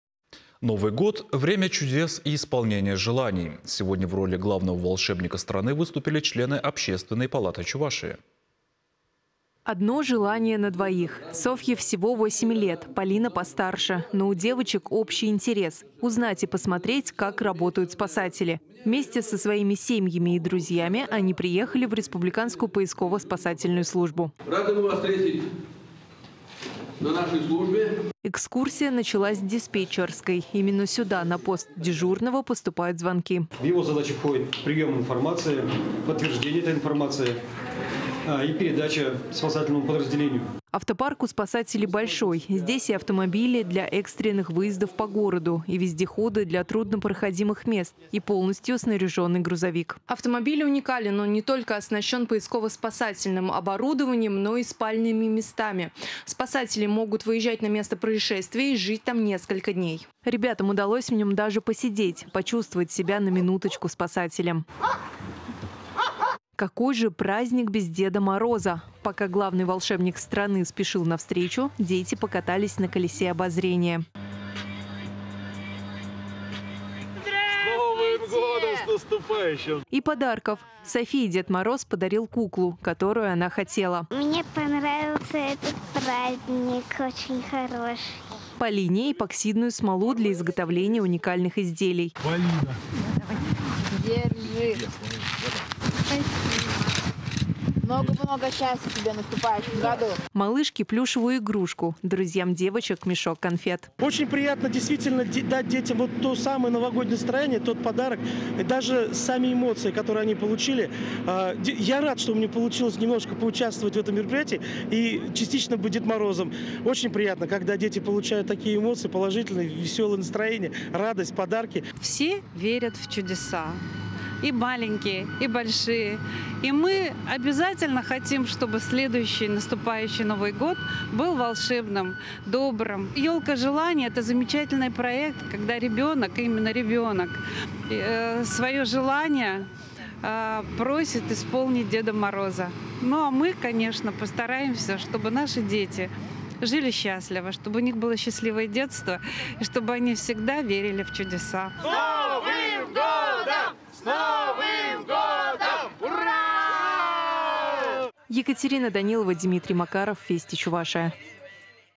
Члены Общественной палаты Чувашии исполнили мечты двух девочек
Новый год время чудес и исполнения желаний! Сегодня в роли главного волшебника страны выступили члены Общественной палаты Чувашии.